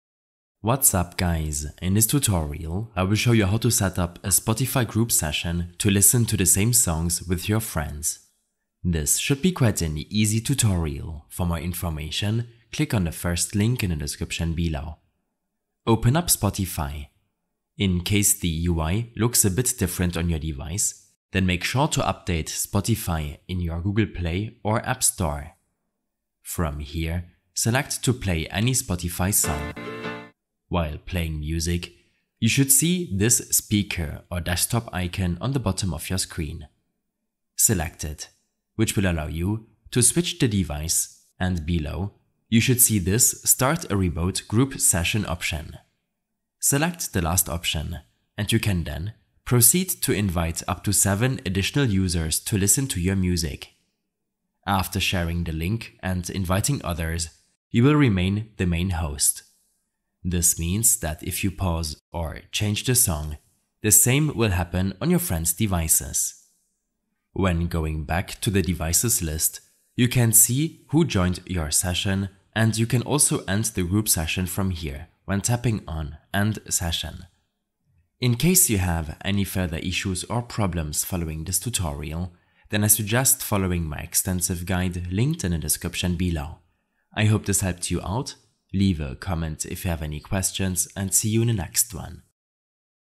How To Setup a Spotify Group Session - Listen with Friends
A short tutorial on how to set up a Spotify group session to listen to the same songs with your friends.

Timestamps:
0:00 Introduction
0:15 Start Spotify Song
0:31 Speaker Icon
0:46 Start a Remove Session
0:54 Share Link
1:07 End Session